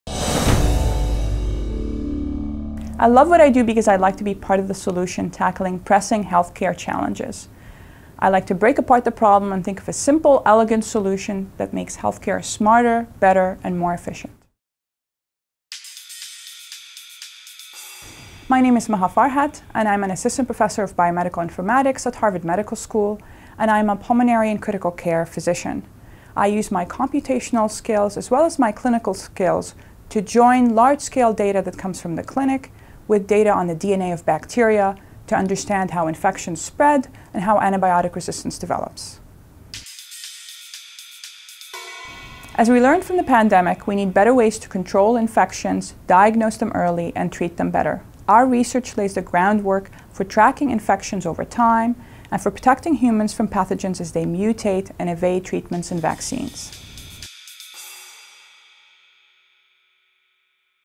Maha Farhat | DNA of Bacteria and Antibiotic Resistance
How computation can fight the spread of infection